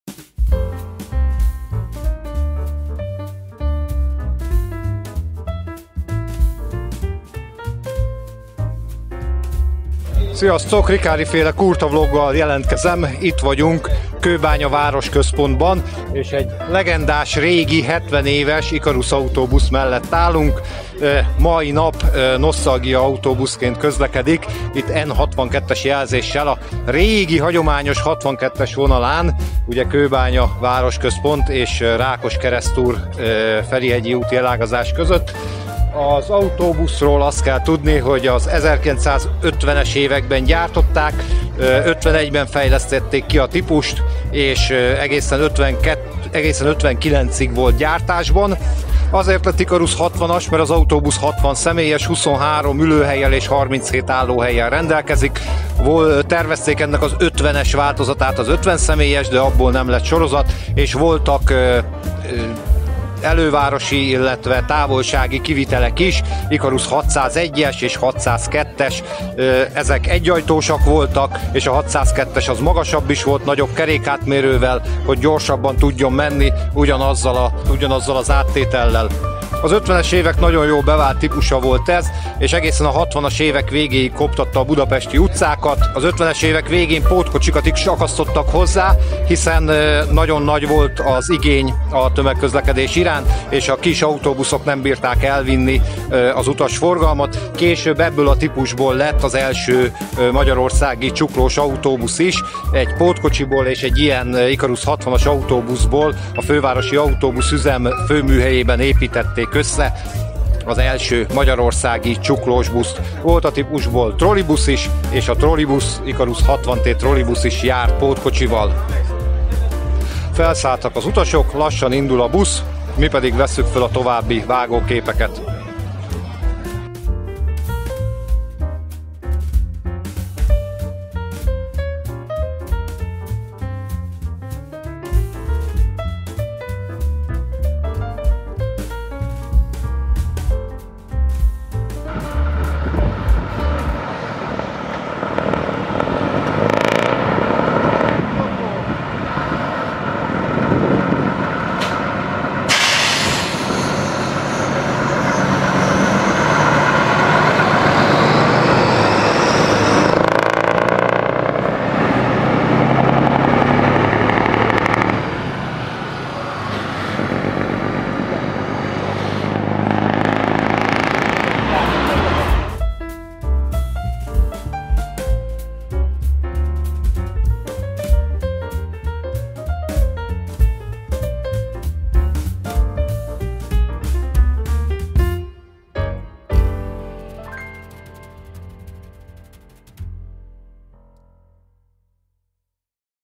70 ÉVES nosztalgia BUSZ közlekedett Budapesten * Kurtavlog
A BKV Ikarus 60-as nosztalgiaautóbuszát csíptük el utasforgalomban, Kőbányán.
Archív képek: Fortepan / Magyar Rendőr, Péterffy István, UVATERV, Farnadi Zsolt
Képszámok: 65334, 142505, 80075, 65257, 24056